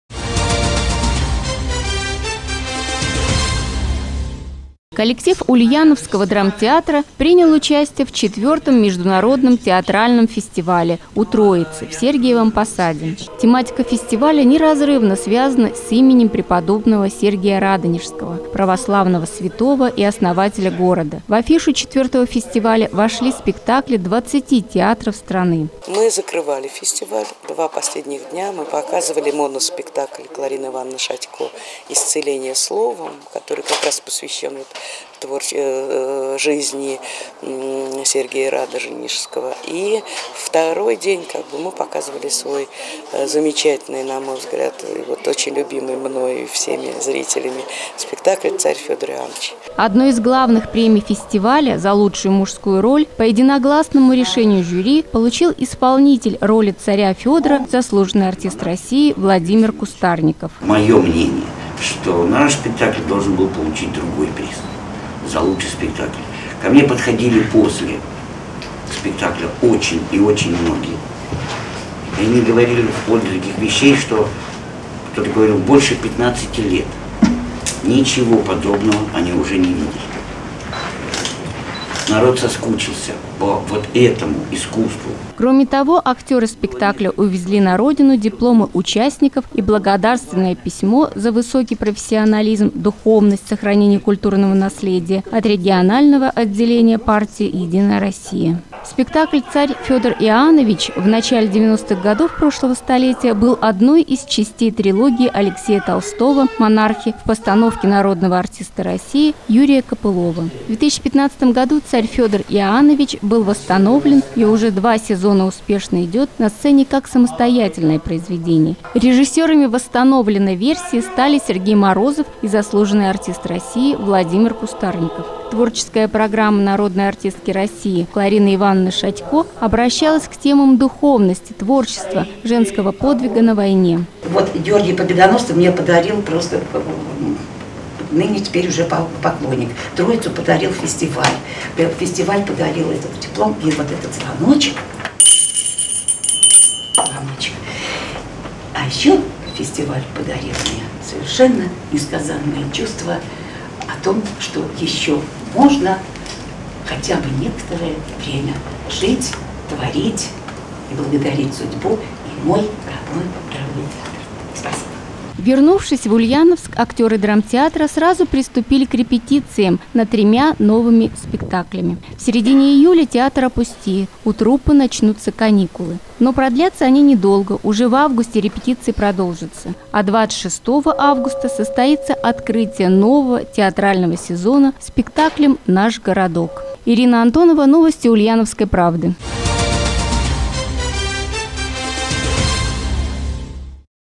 Владимир Кустарников получил премию за «царскую» роль
Ульяновский драматический театр имени И.А.Гончарова вернулся с фестиваля «У Троицы» с наградами.